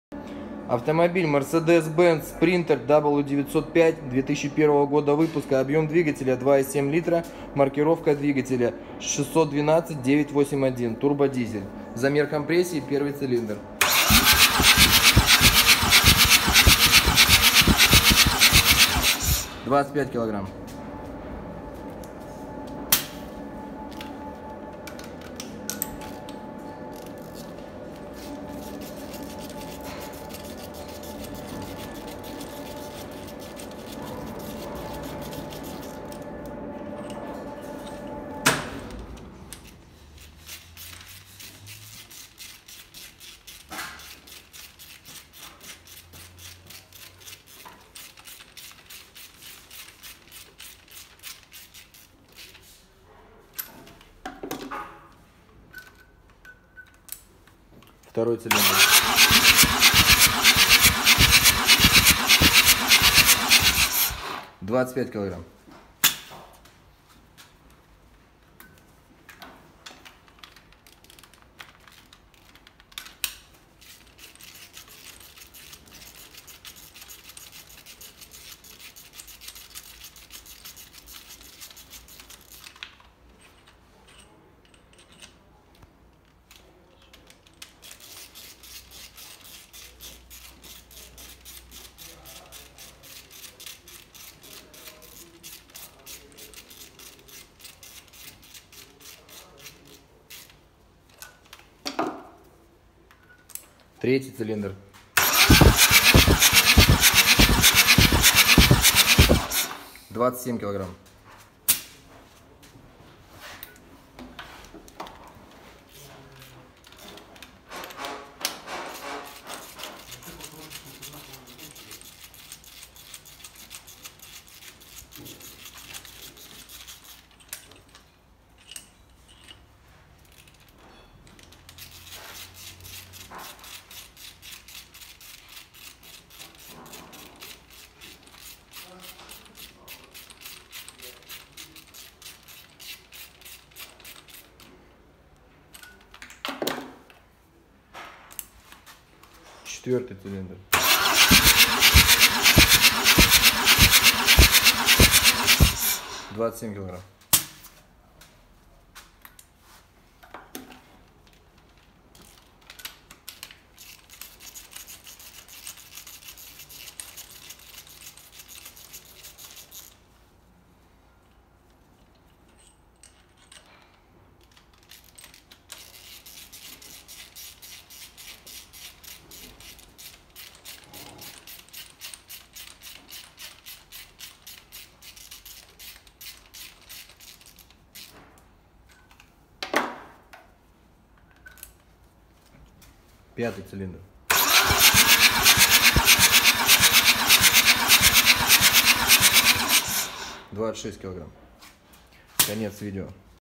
Двигатель Mercedes Benz для Sprinter (901-905)/Sprinter Classic
2.7Л. КОМПРЕССИЯ: 1Ц:25 2Ц:25 3Ц:27 4Ц:27 5Ц:26 OM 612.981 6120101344